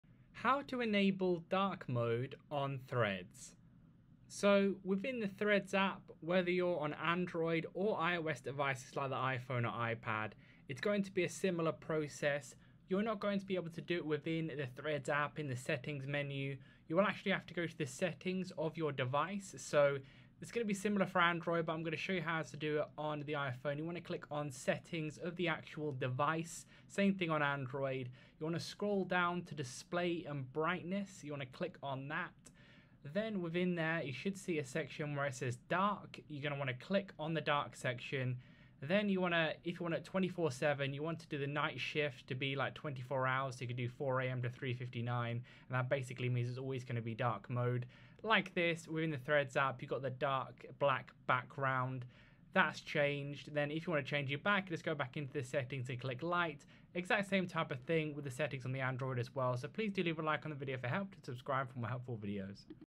How to Enable Dark Mode on Threads
How to Enable Dark Mode on Threads. You can turn on dark mode with android or ios devices like the iphone or ipad. Also, you can use a black background from instagram threads on your laptop computer with the new threads instagram app update in 2023.